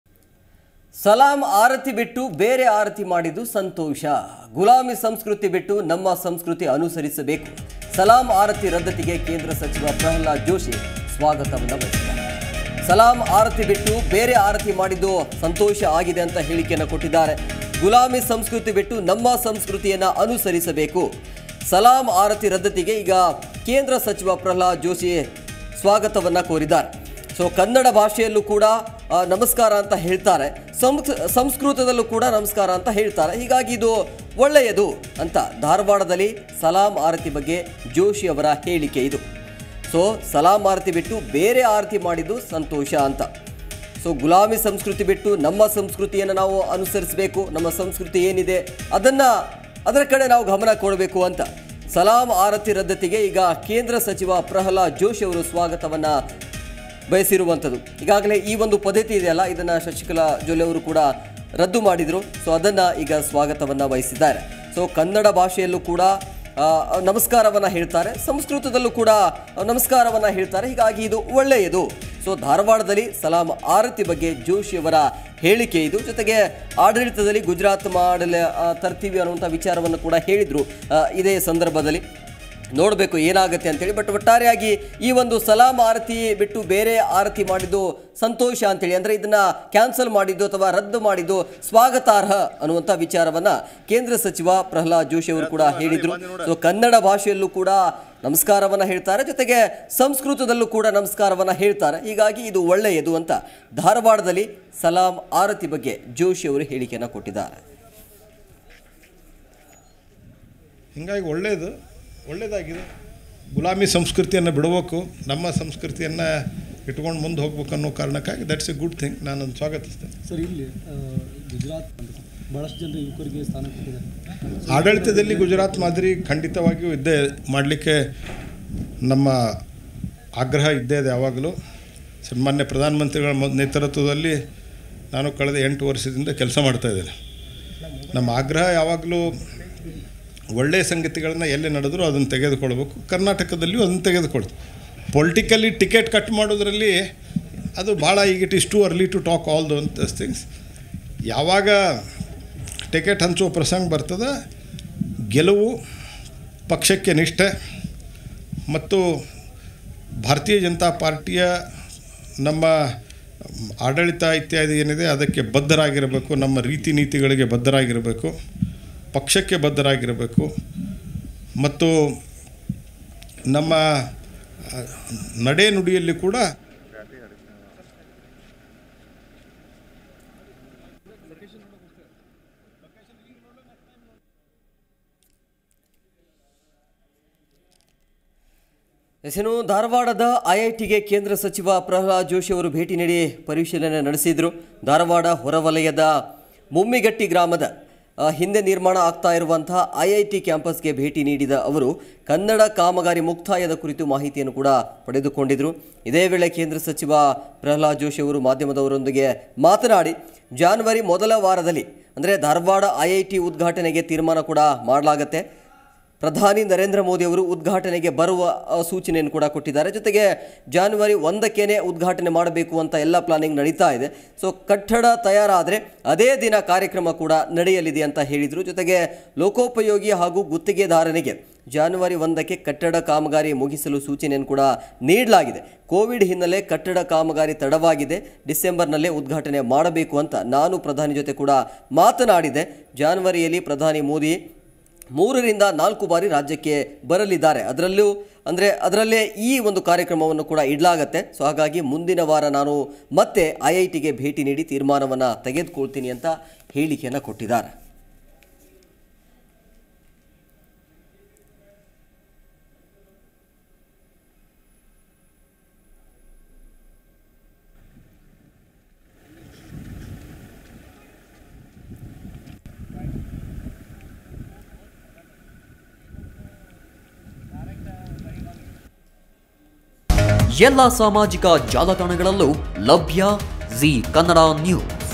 Prahalad Joshi | ಸಲಾಂ ಆರತಿ ರದ್ದತಿಗೆ ಜೋಶಿ ಸ್ವಾಗತ
ಸಲಾಂ ಆರತಿ ಬಿಟ್ಟು ಬೇರೆ ಆರತಿ ಮಾಡಿದ್ದು ಸಂತೋಷ. ಗುಲಾಮಿ ಸಂಸ್ಕೃತಿ ಬಿಟ್ಟು, ನಮ್ಮ ಸಂಸ್ಕೃತಿ ಅನುಸರಿಸಬೇಕು. ಸಲಾಂ ಆರತಿ ರದ್ದತಿಗೆ ಕೇಂದ್ರ ಸಚಿವ ಪ್ರಹ್ಲಾದ್‌ ಜೋಶಿ ಸ್ವಾಗತ ಅಂತಾ ಕೇಂದ್ರ ಸಚಿವ ಪ್ರಹ್ಲಾದ್‌ ಜೋಶಿ ಹೇಳಿದ್ದಾರೆ.